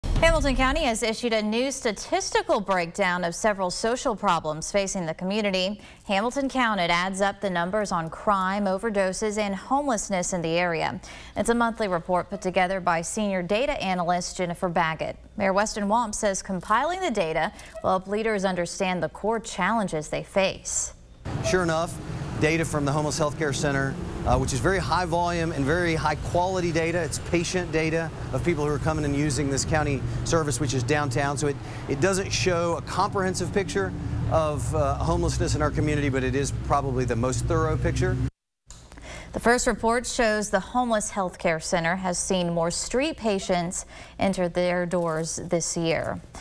"Hamilton Counted" breaks down social problems in the community
Hamilton County has issued a new statistical breakdown of several social problems facing the community.